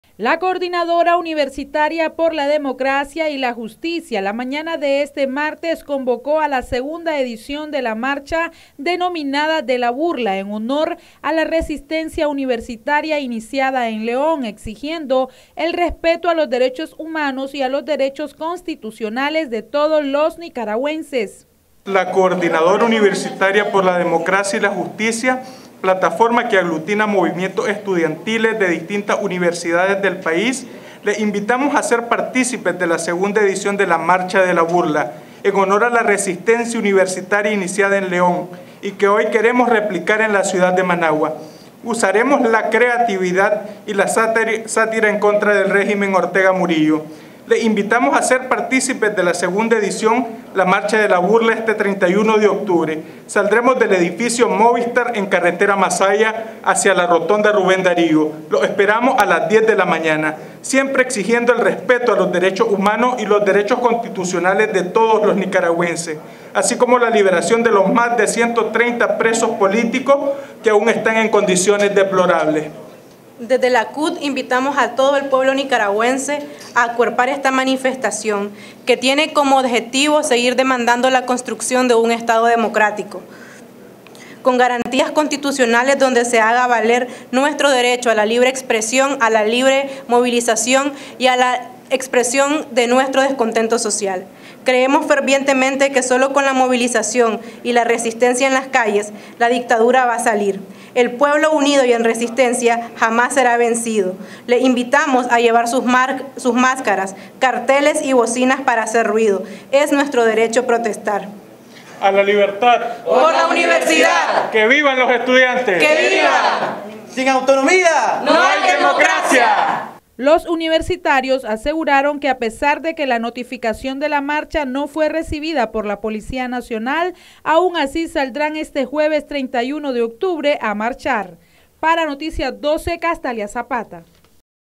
UNIVERSITARIOS LLAMAN A MARCHA DE LA BURLA PARA ESTE 31 DE OCTUBRE
Universitarios llaman a marcha denominada ¨de la burla¨ para este 31 de octubre